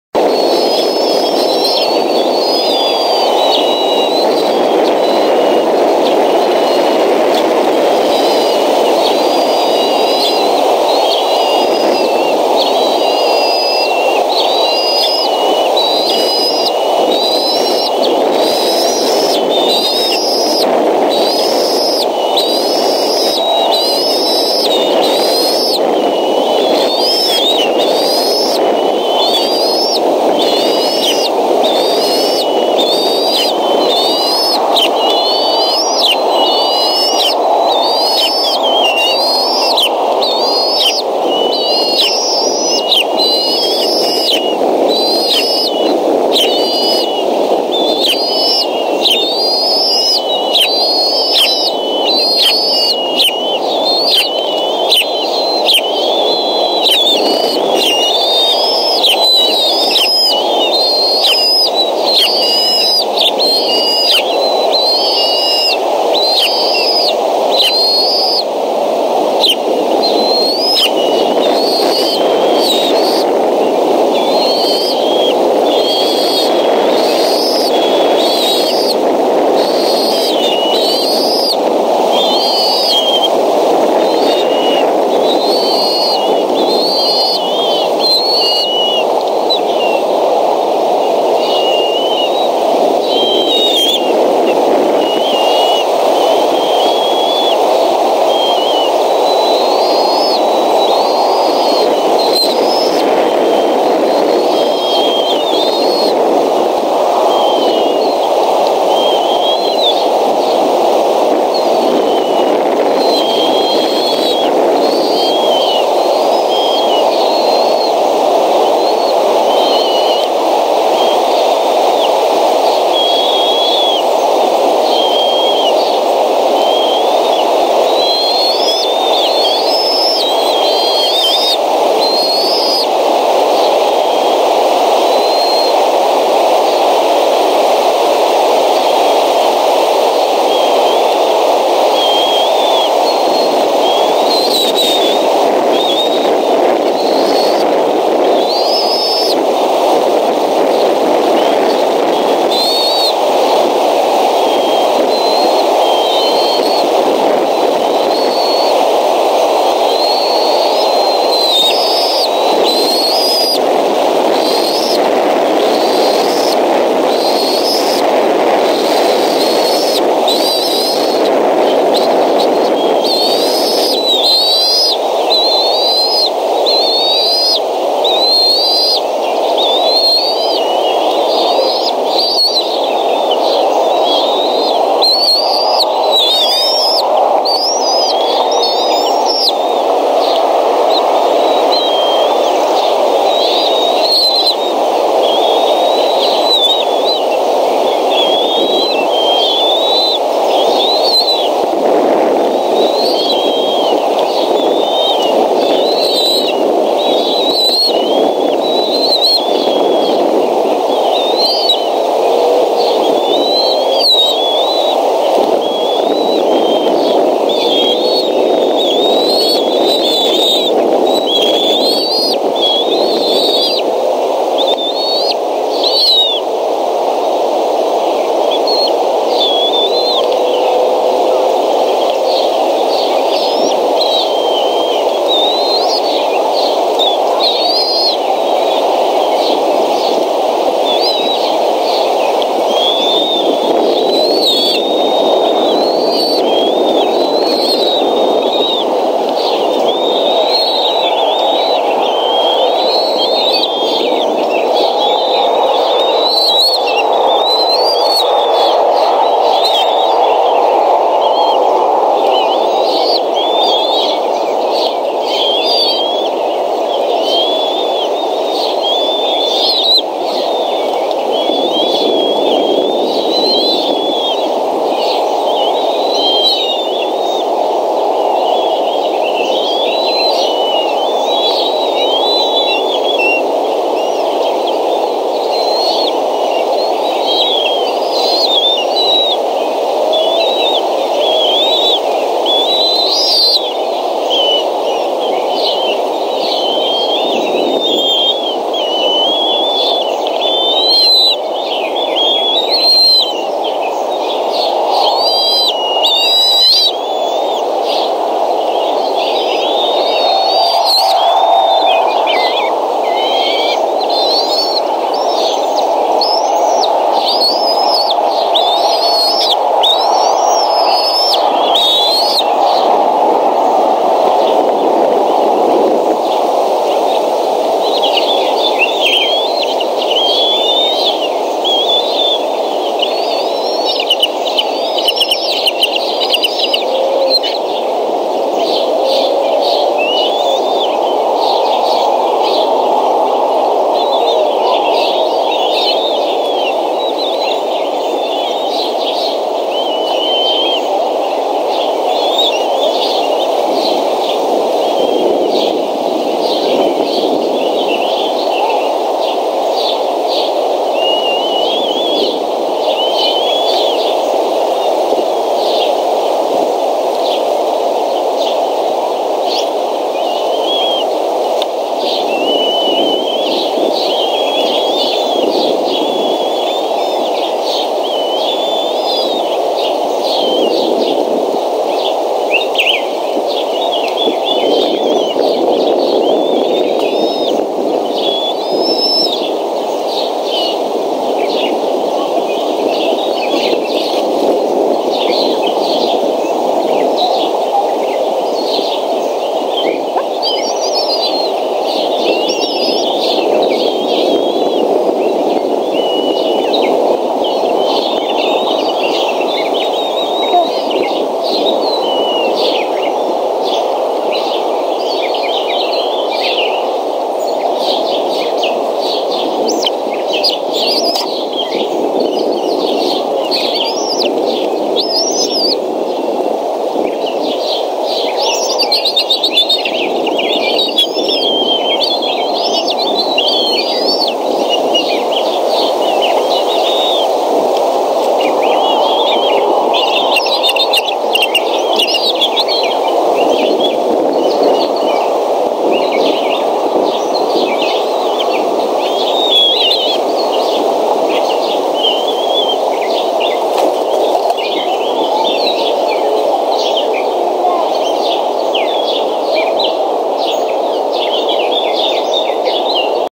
Mom Decorah home with fish D12 upfront,27/5/2012
Sunday 4:22pm CDT,not sure of the other to eaglets lol.

raptorresource